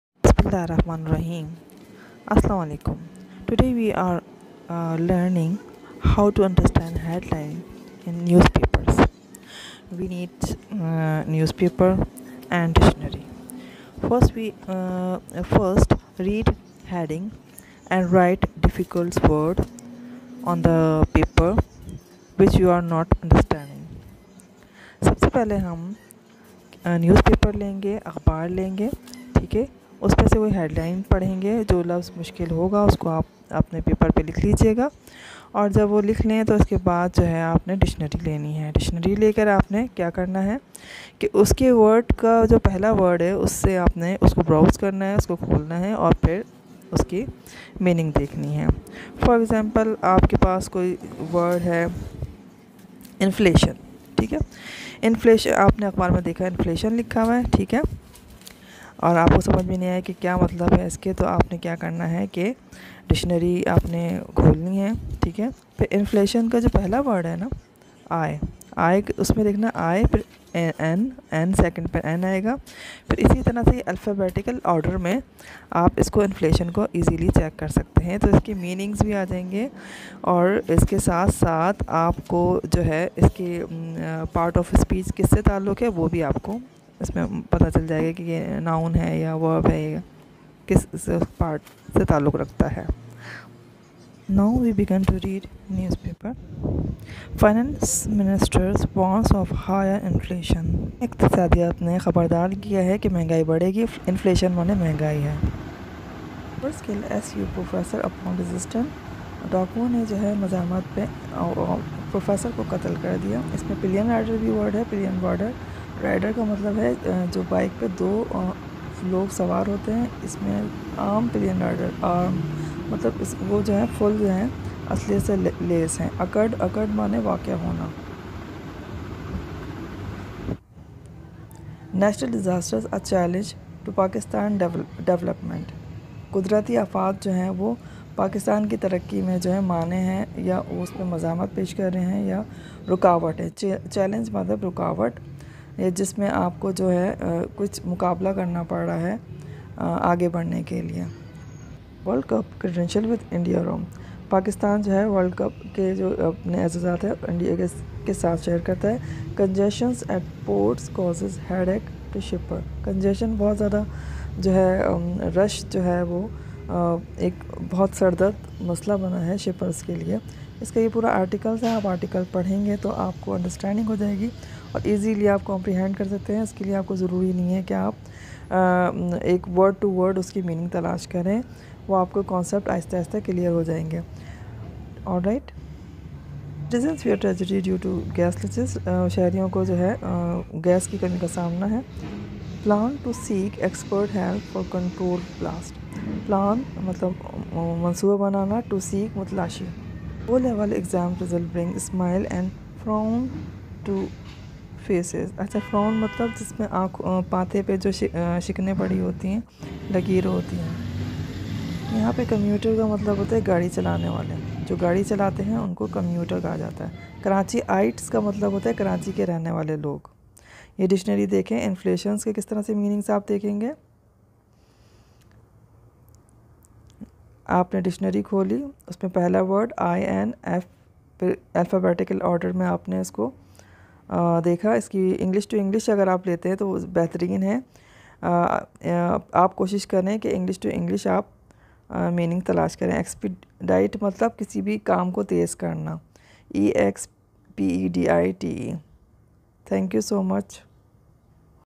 How to Understand English Newspapers | Newspapers headings
Description:  video is depicting, how to read English newspapers with help of a dictionary.
Reading newspapers will help you to learn English easily.

easily comprehend the news, with help of a dictionary.
Easy way to comprehend advanced English vocabularies -

Verbs | actions | continues tenses | Urdu | Hindi